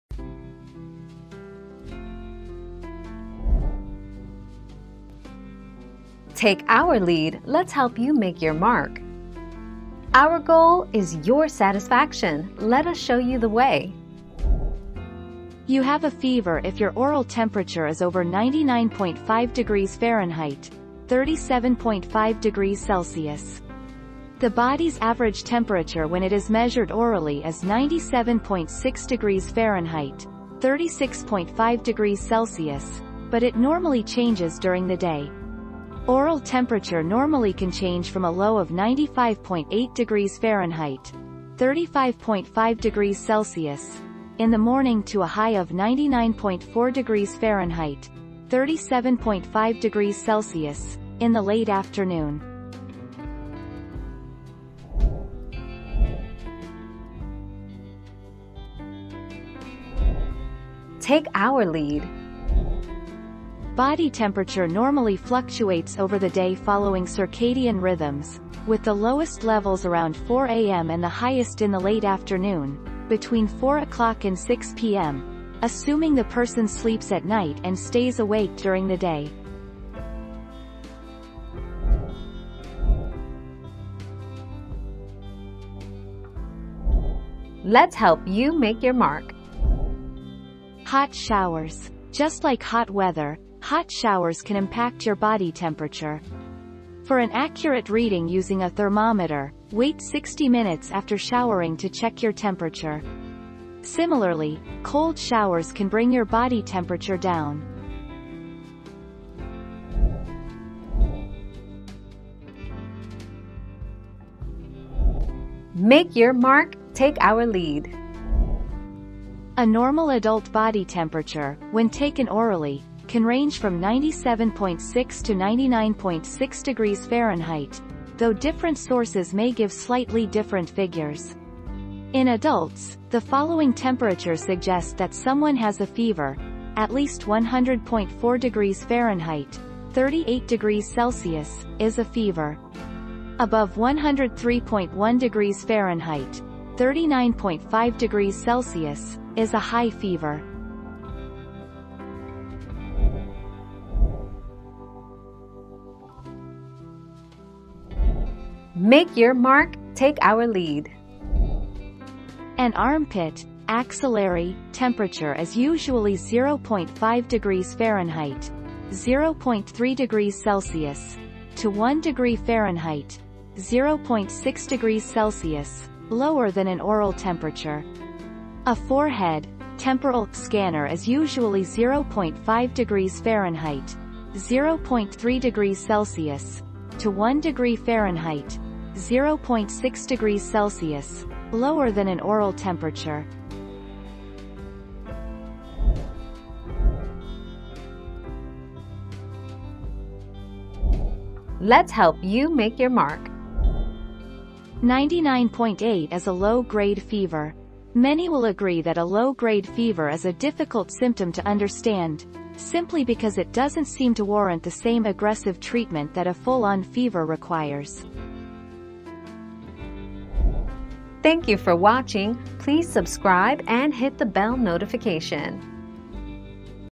Is 99.4 a fever?
00:00 - Is 99.4 a fever?
00:56 - What time of the day is your body temperature highest?
01:23 - Will a shower raise your temperature?
01:51 - Is a 99.2 temp normal?
02:34 - How accurate is armpit temp?
03:17 - Is 99.8 degrees a fever?

Laura S. Harris (2021, April 3.) Is 99.4 a fever?
    AskAbout.video/articles/Is-99-4-a-fever-208737